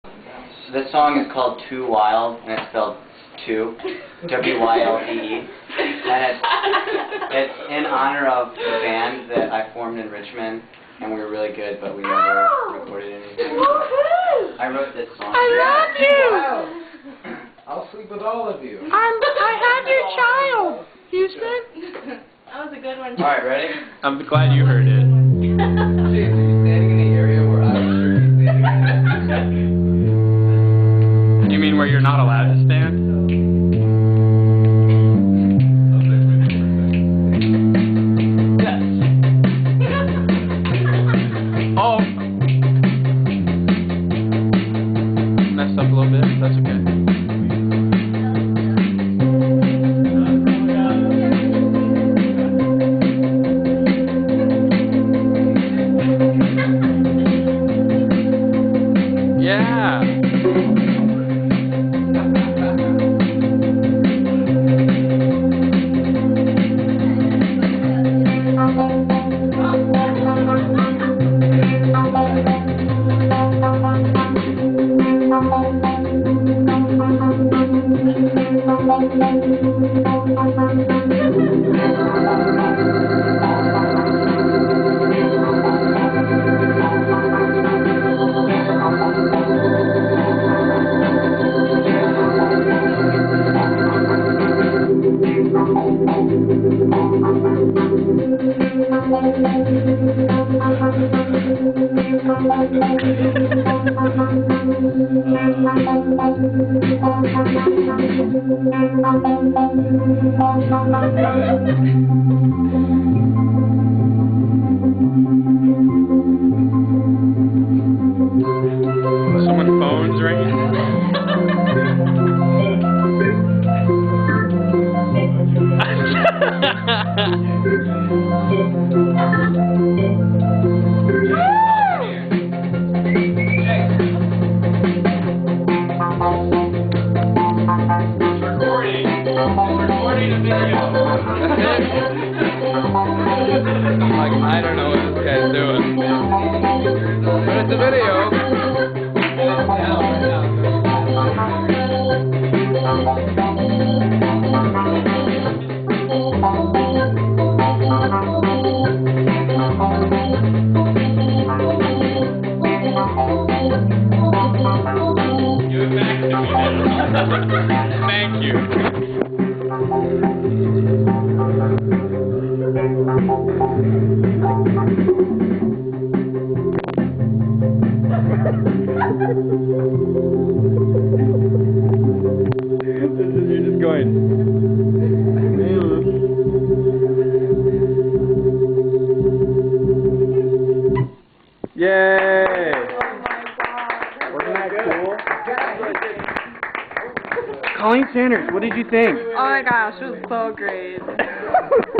Houston's Organ Recital
Houston plays his organ for us.